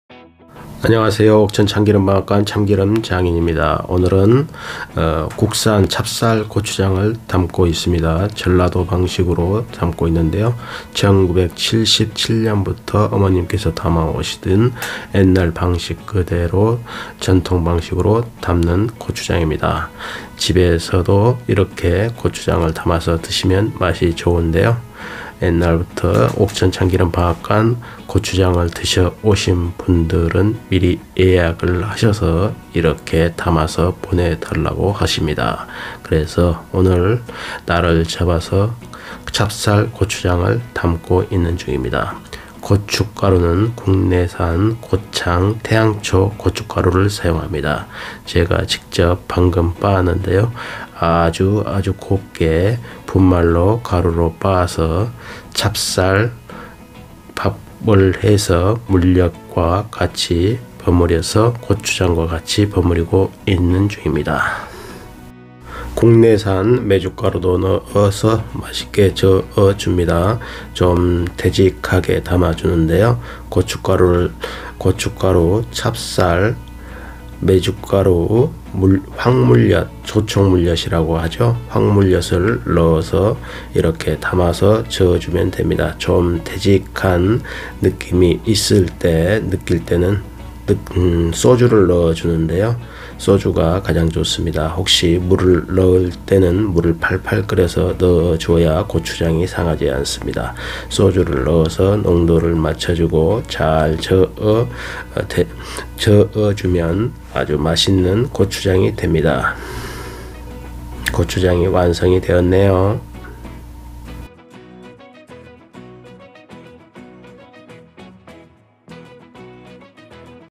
1977년 옛날 전통방식 전라도 국산 찹쌀고추장 예약 담았어요
1977년부터 방앗간을 해오면서 지금까지 옛날 전통방식으로 담아오고 있습니다
모든 재료는 100% 국산(국내산)입니다
전라도 찹쌀 고추장은 특히 찰지고 윤기가 자르르 흘러
숙성되면 될수록
쫀득 쫀득한 찰지고 윤기가 좋아 맛 좋기로 유명하죠
1년에 한번 담아 한 해를 먹는 고추장
많이 담으면 2년도 드시는 분들이 많습니다
보관은 잘 숙성된 후 냉장 보관이 좋습니다